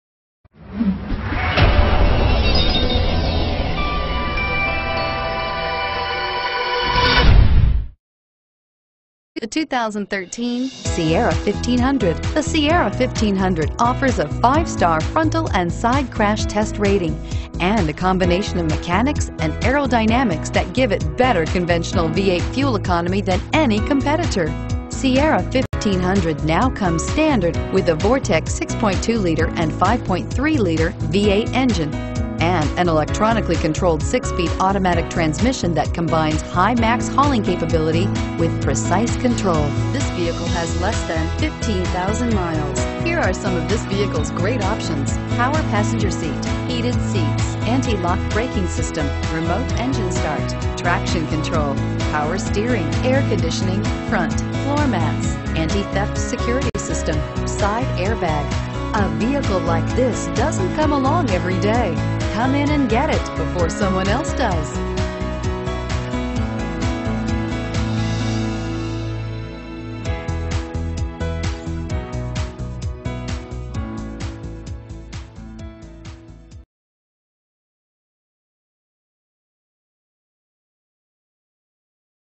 2013 GMC SIERRA 1500 Norfolk NE 5463A
2013 GMC SIERRA 1500

You'll love this 2013 GMC SIERRA 1500. This is a car you'll want to take home. With 14245 miles, it features Automatic transmission and an exterior color of BLACK/PEWTER. Call and get in touch with Norfolk GM Auto Center directly, and be the first to open the car door today!